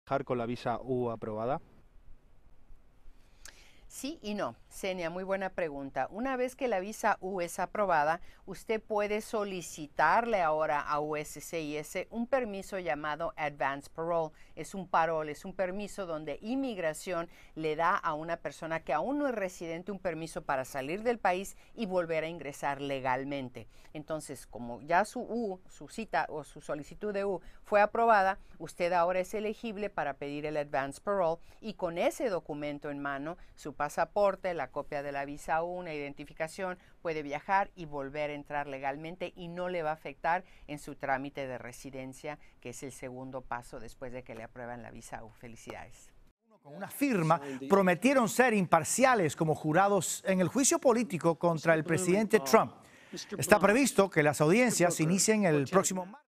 ¿Puedo viajar con la visa U aprobada? | Noticias Telemundo
Video oficial de Noticias Telemundo. Puede viajar con la visa U al solicitar el "advance parole" el cual permite salir y entrar de EE. UU. legalmente.